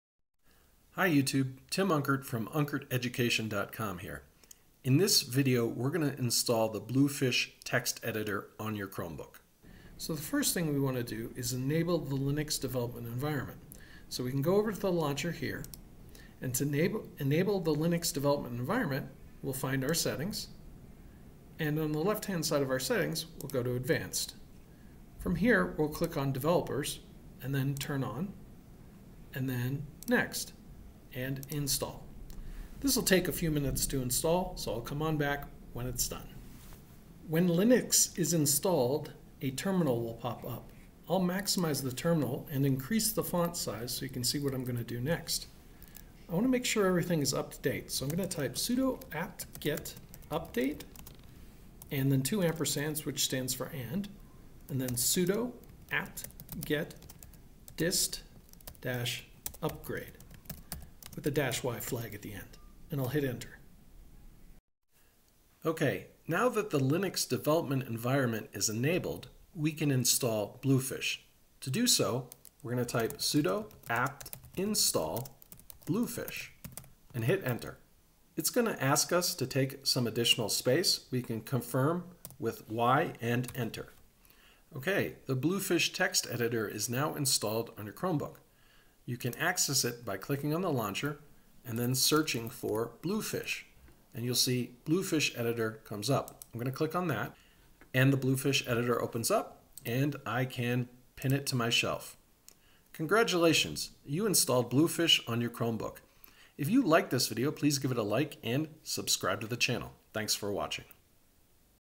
How to Install Bluefish on a Chromebook
In this video, I'll show you how to install Bluefish on a Chromebook.